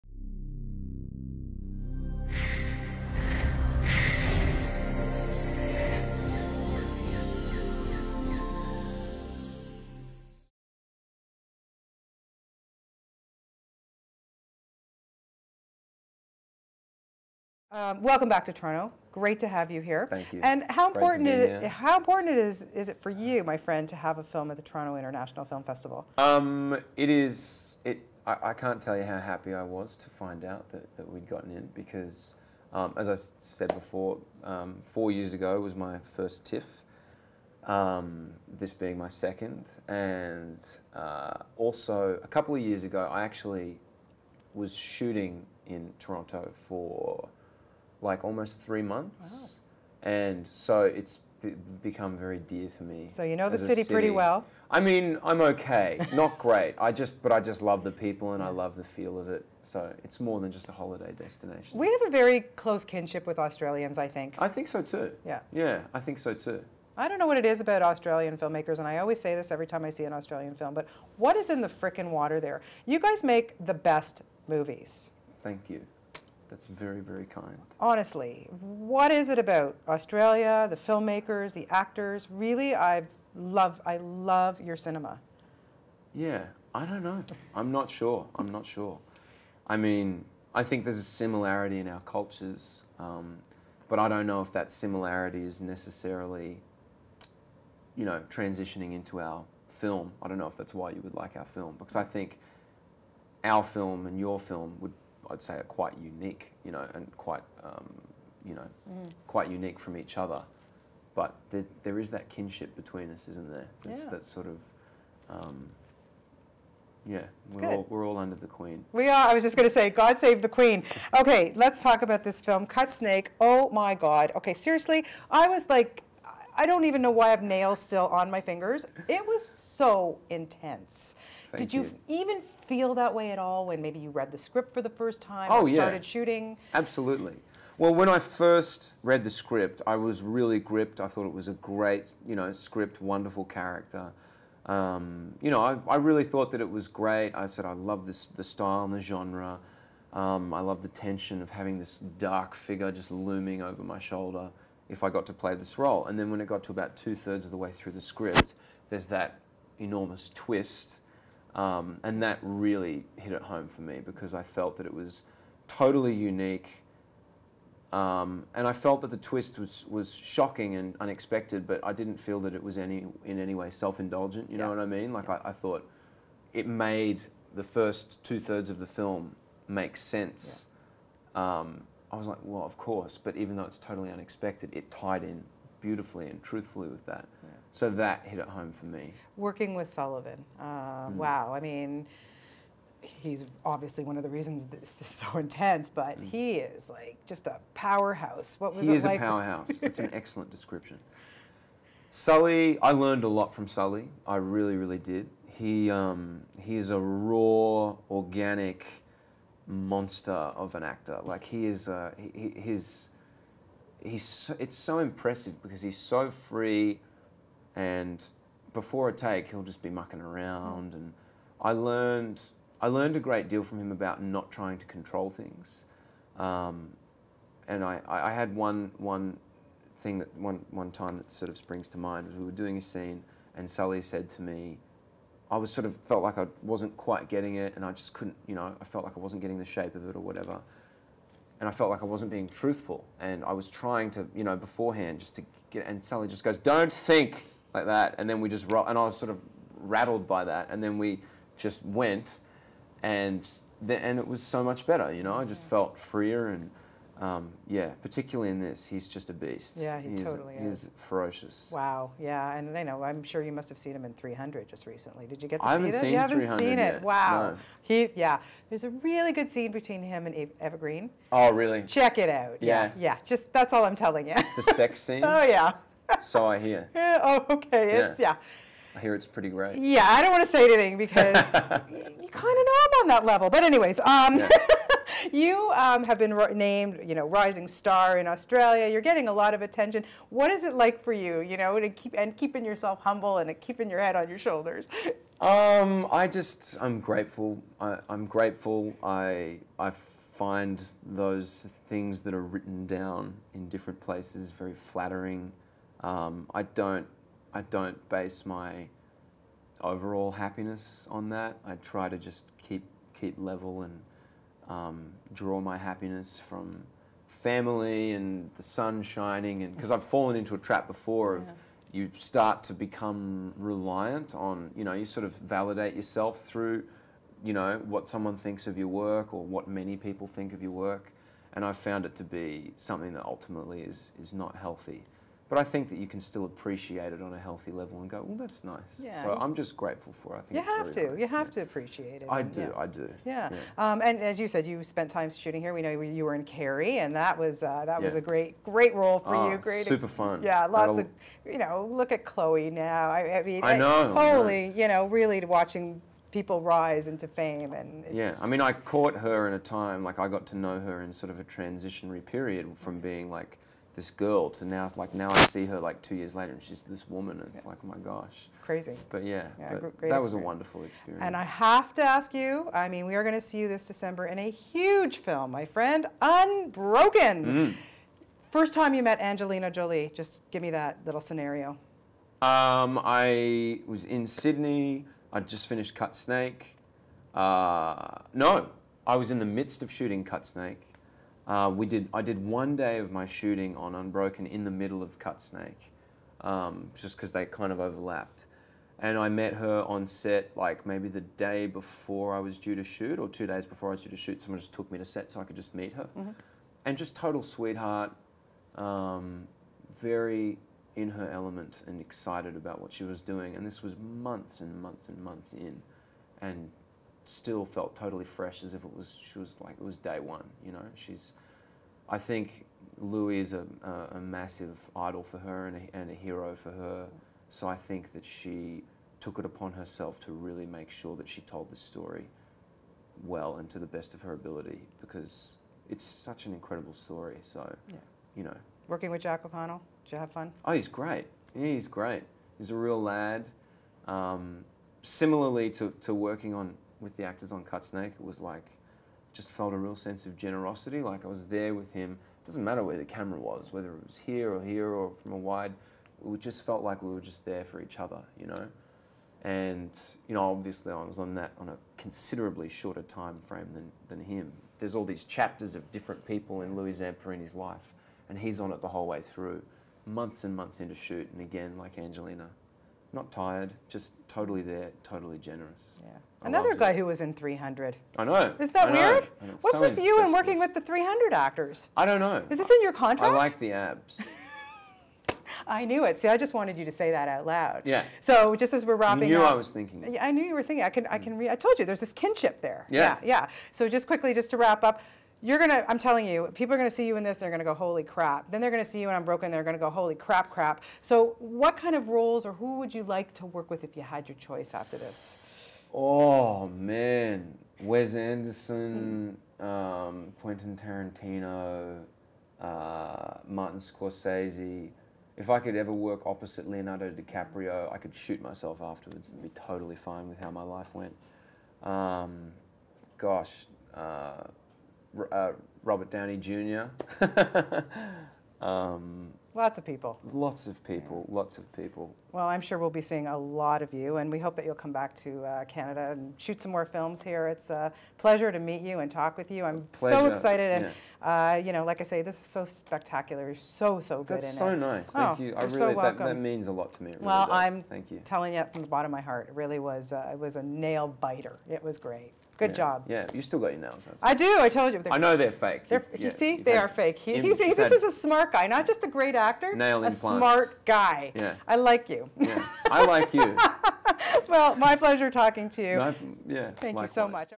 Alex Russell Interview for Cutsnake
Up and coming Australian actor Alex Russell talks about his role in Cutsnake and the upcoming, Unbroken ( directed by Angelina Jolie)